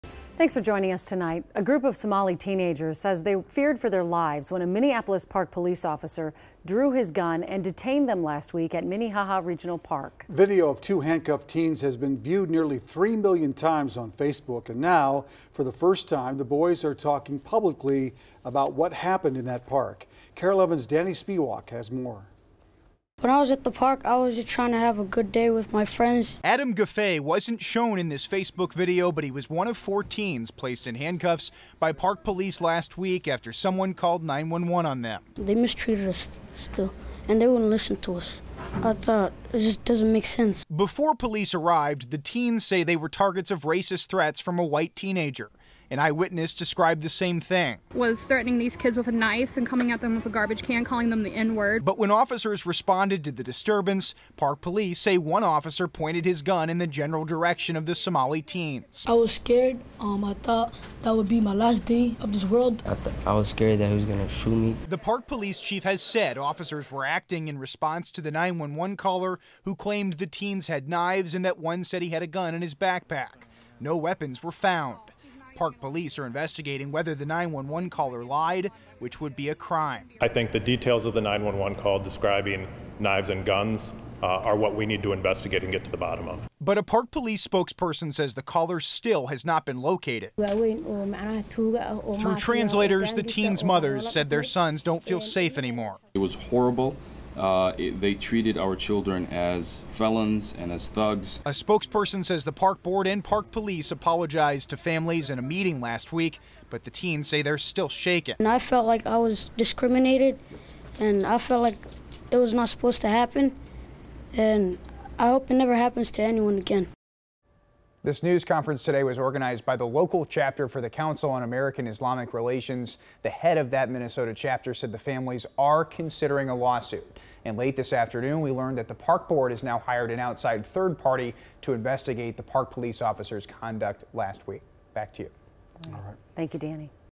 Somali teenagers say they feared for their lives at Minnehaha Park
Somali teenagers said Monday they feared for their lives when Park Police pointed a weapon at them and detained them at Minnehaha Regional Park last week.